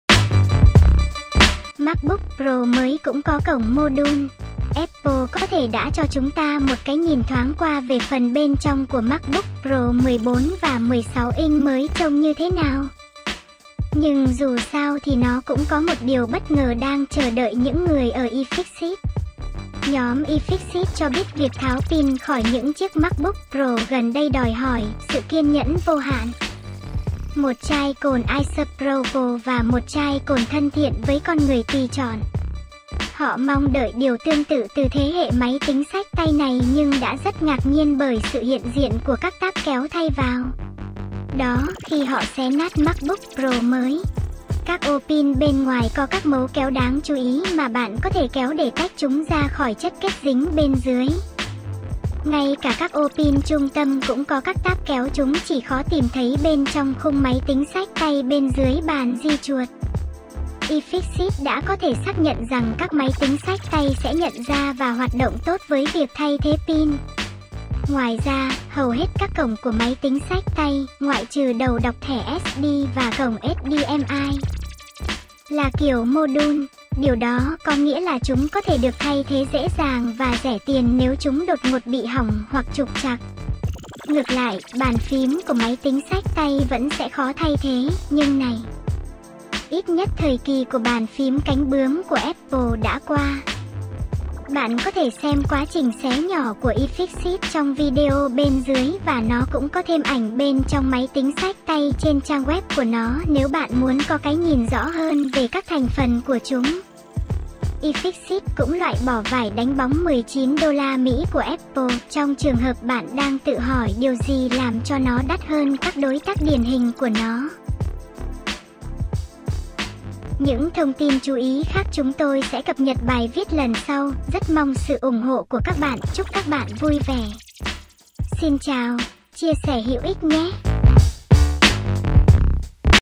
MacBook Pro tiết lộ pin dễ tháo lắp
Apple có thể đã cho chúng ta một cái nhìn thoáng qua về phần bên trong của MacBook Pro 14 và 16 inch mới trông như thế nào, nhưng dù sao thì nó cũng có một điều bất ngờ đang chờ đợi những người ở iFixit.

Nhóm iFixit cho biết việc tháo pin khỏi những chiếc MacBook Pro gần đây đòi hỏi "sự kiên nhẫn vô hạn, một chai cồn isopropyl và một chai cồn thân thiện với con người tùy chọn." Họ mong đợi điều tương tự từ thế hệ máy tính xách tay này nhưng đã rất ngạc nhiên bởi sự hiện diện của các tab kéo thay vào đó khi họ xé nát MacBook Pro mới.

Các ô pin bên ngoài có các mấu kéo đáng chú ý mà bạn có thể kéo để tách chúng ra khỏi chất kết dính bên dưới. Ngay cả các ô pin trung tâm cũng có các tab kéo - chúng chỉ khó tìm thấy bên trong khung máy tính xách tay bên dưới bàn di chuột. iFixit đã có thể xác nhận rằng các máy tính xách tay sẽ nhận ra và hoạt động tốt với việc thay thế pin.

Ngoài ra, hầu hết các cổng của máy tính xách tay, ngoại trừ đầu đọc thẻ SD và cổng HDMI, là kiểu mô-đun. Điều đó có nghĩa là chúng có thể được thay thế dễ dàng và rẻ tiền nếu chúng đột ngột bị hỏng hoặc trục trặc. Ngược lại, bàn phím của máy tính xách tay vẫn sẽ khó thay thế, nhưng này, ít nhất thời kỳ của bàn phím cánh bướm của Apple đã qua .